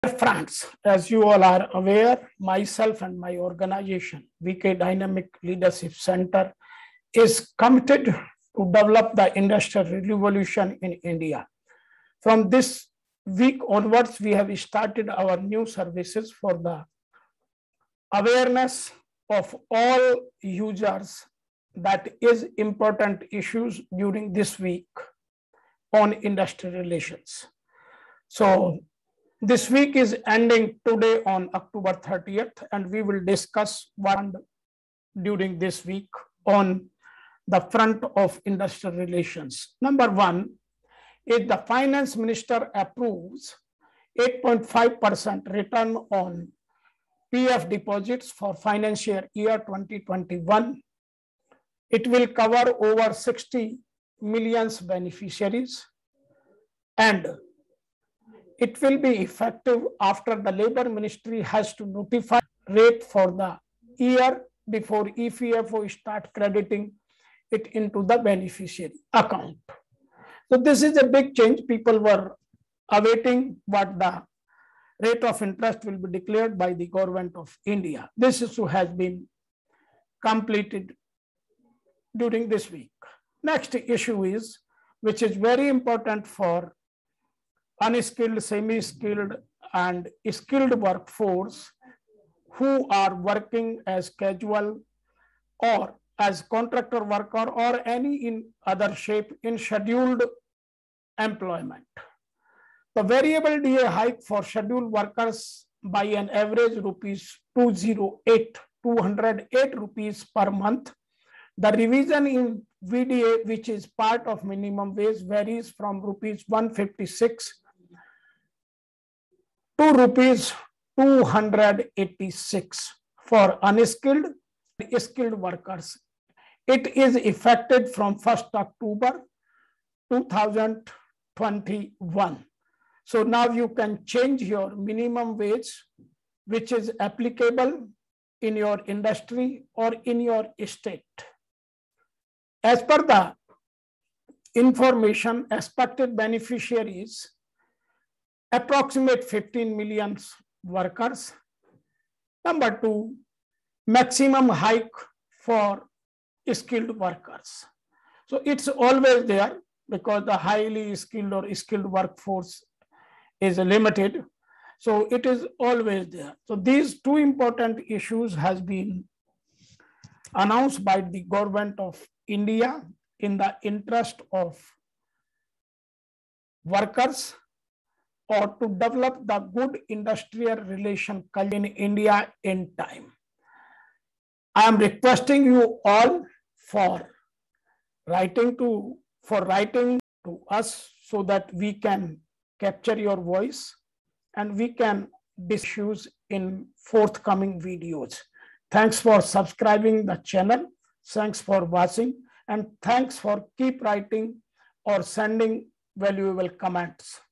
Important issues during this week - Oct 30, 2021
What important issues have been discussed, amendments have been made and legal orders passed by various high courts and the Apex court of India.